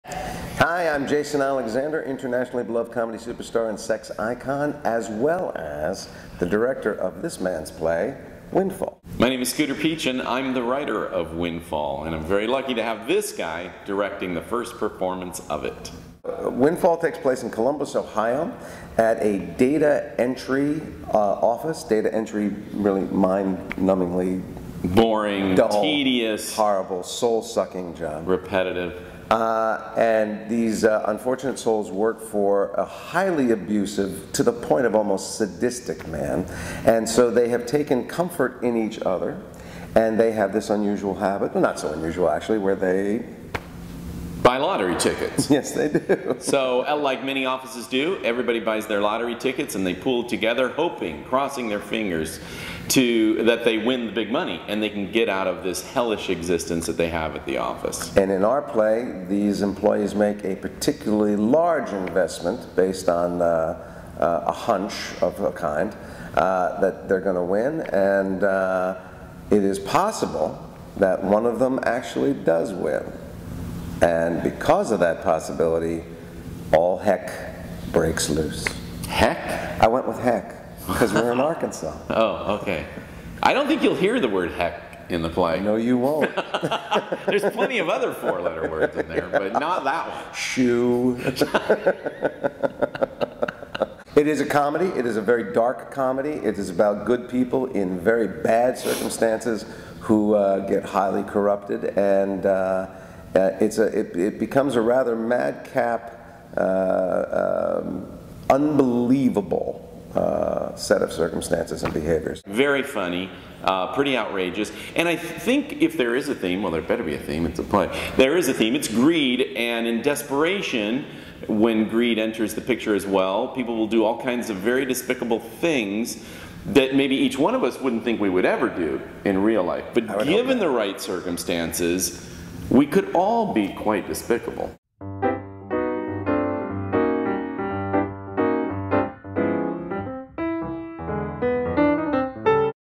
From Script to Stage | The Plot of Windfall
Windfall,world premiere
Arkansas Repertory Theatre

Director Jason Alexander and Playwright Scooter Pietsch explain the plot of this world premiere production and why they think Arkansas audiences need to experience it.